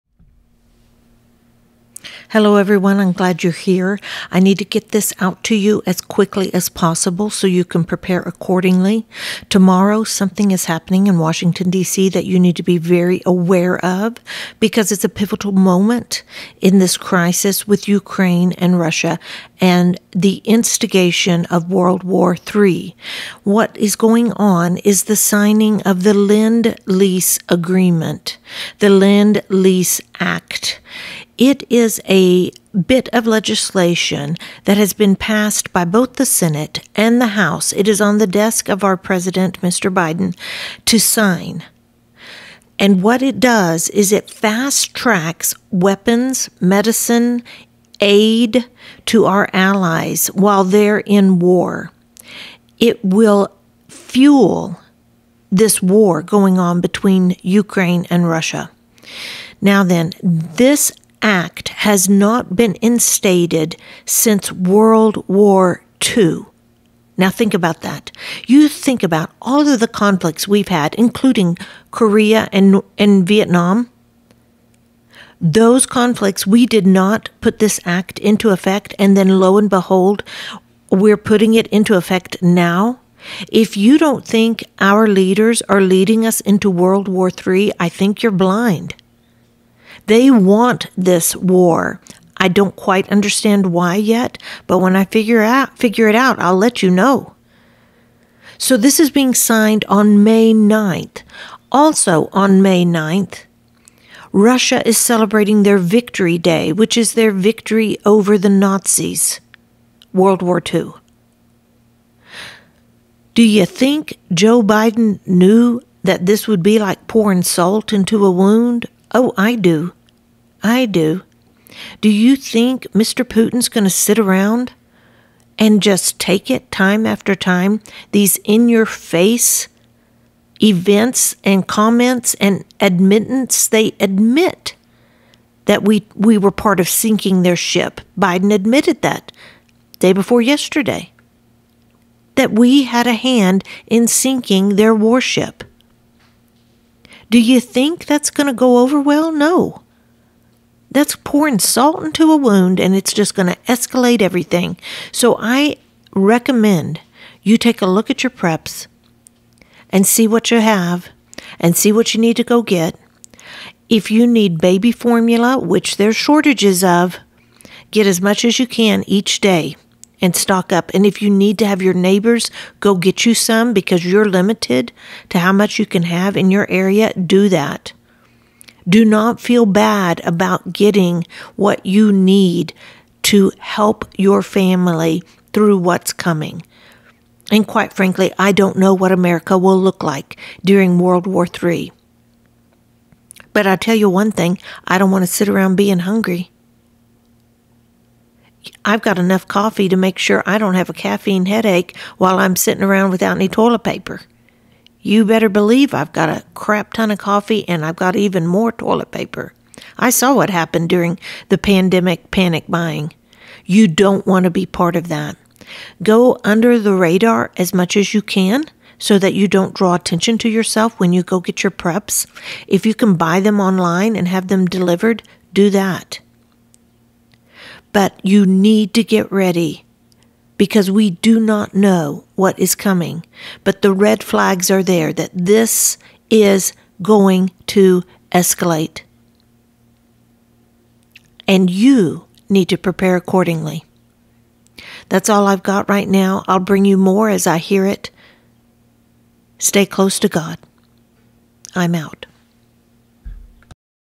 NEWS ALERT! NUCLEAR WAR RED FLAG! WW3 IS NEAR! Respond accordingly
The lend lease act is being passed! This is a huge red flag the war will escalate!